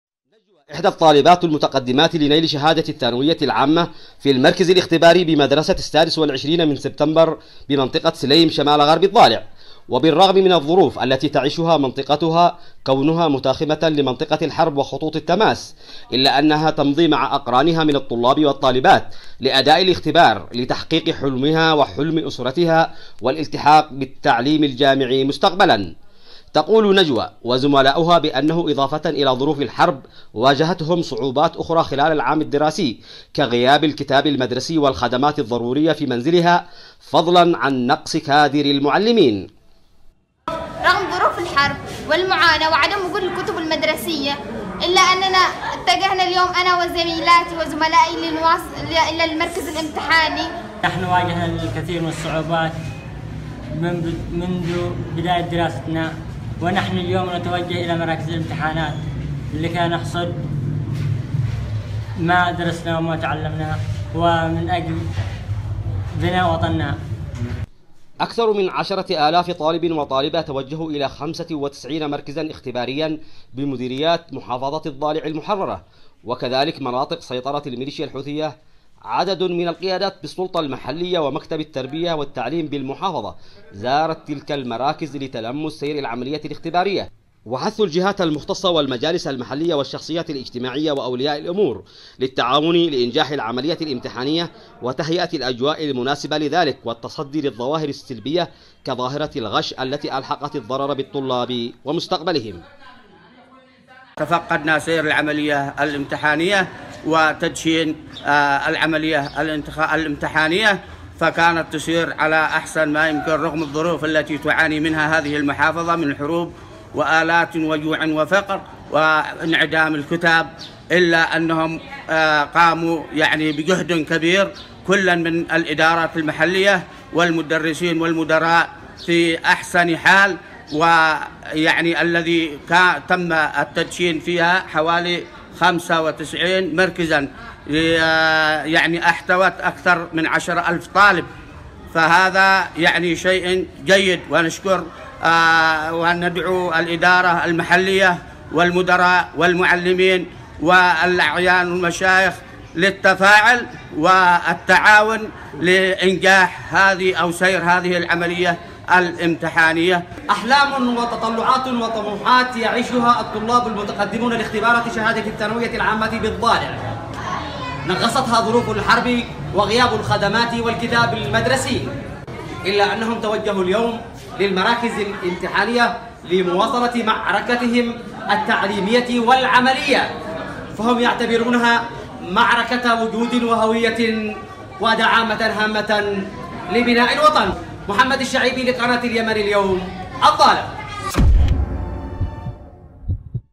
الضالع | المتقدمون لاختبارات الثانوية يتحدون الحرب وغياب الكتاب والمعلم
الضالع .. ‏احلامٌ وتطلعاتٌ يعيشُها الطلابُ ‏المتقدمون لامتحانات الثانويةِ العامةِ .. تنغصها ‏ظروفُ الحربِ وعدم ُتوفرِ الكتابِ المدرسي وغياب المعلم .. إلا أن كلَّ ذلك لم يثنْهم عن مواصلةِ معركتِهم العلميةِ بتحدٍ واصرارْ

تليجرام